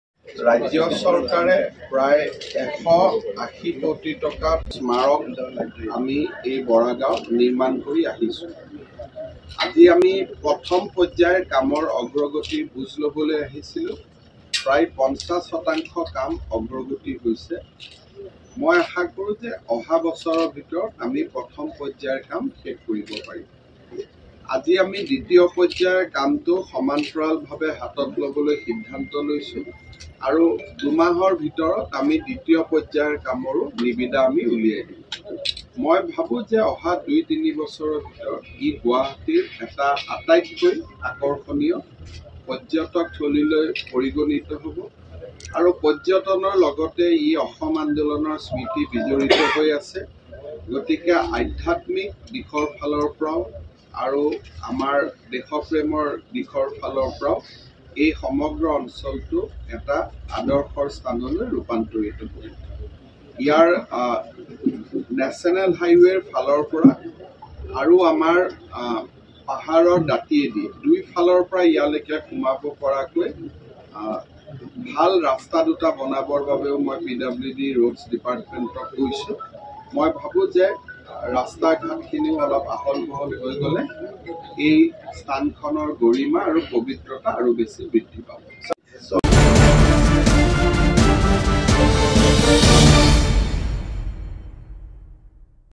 ১৮০ কোটি টকা ব্যয় সাপেক্ষে শ্বহীদ স্মাৰক ক্ষেত্ৰৰ কাম চলিছে
🔴প্ৰথম পৰ্য্যায়ৰ ৫০% কাম সম্পন্ন হৈছে
🔴অহা দুই তিনিবছৰৰ ভিতৰত ৰাইজৰ বাবে উলিয়াই দিব ক্ষেত্ৰখন
🔴শ্বহীদ শ্মাৰকত ১৩ মহলীয়া এটা অট্টালিকা থাকিব